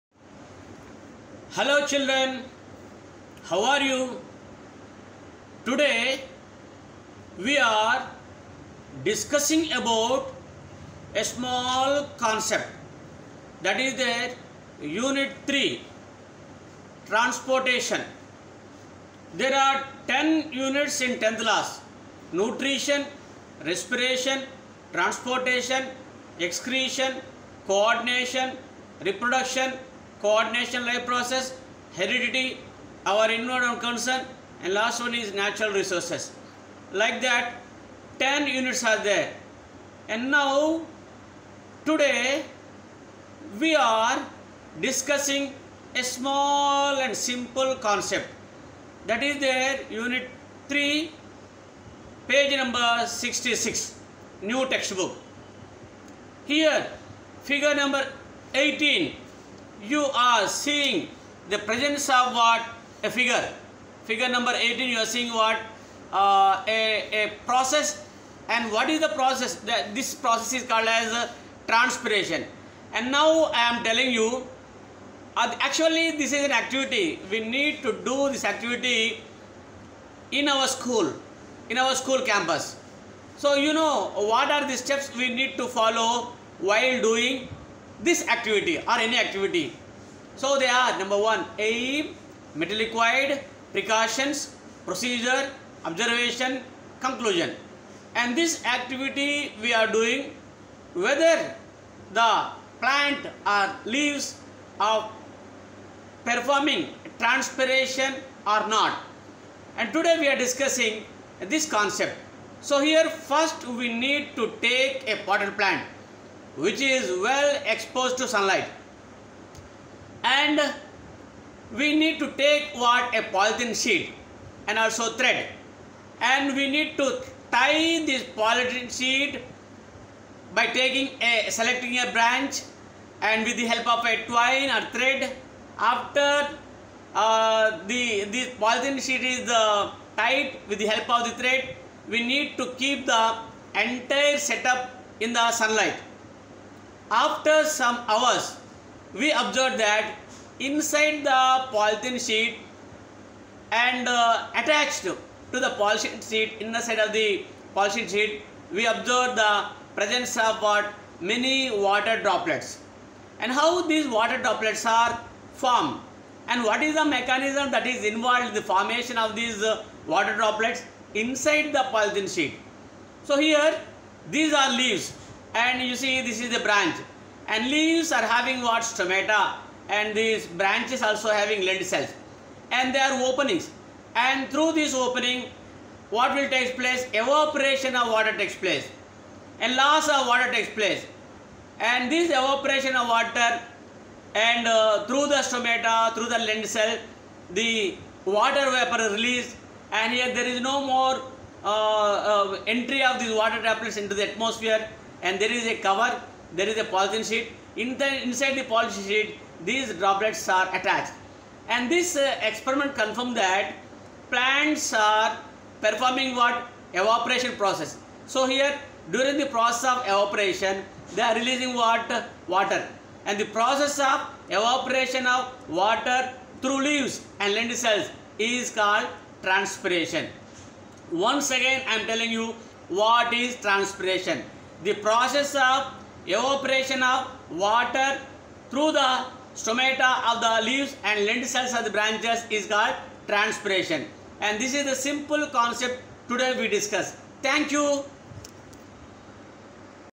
Banala jaya raj sir explaining about Transpiration process in plants. class-x.
BANALA JAYA RAJ SIR EXPLAINING TRANSPIRATION PROCESS. CLASS-X
EVAPORATION OF THROUGH LEAVES IS TRANSPIRATION. COMPLETE DESCRIPTION ABOUT THIS IS GIVEN PAGE NUMBER-66. CLASS-X. EVAPORATION OF WATER IN PLANTS TAKE PLACE THROUGH LEAVES AND LENTICELS.